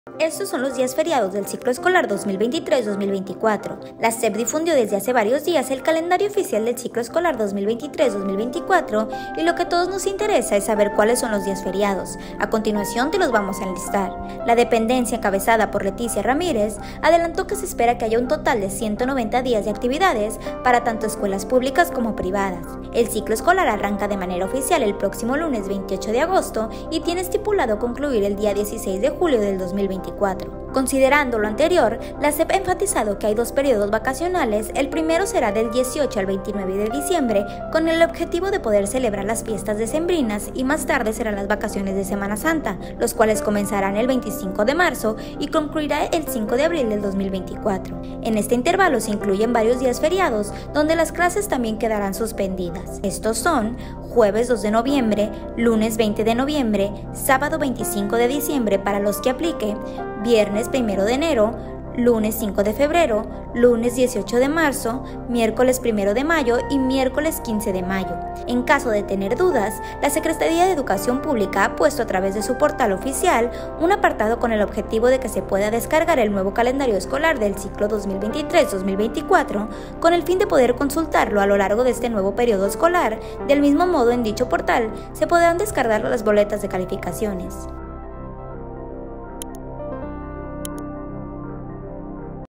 ¿Listo para la escuela? Estos son los días feriados del ciclo escolar 2023 2024
La SEP difundió desde hace varios días el calendario oficial del ciclo escolar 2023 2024 y lo que a todos nos interesa es saber cuáles son los días feriados, por eso te lo contamos.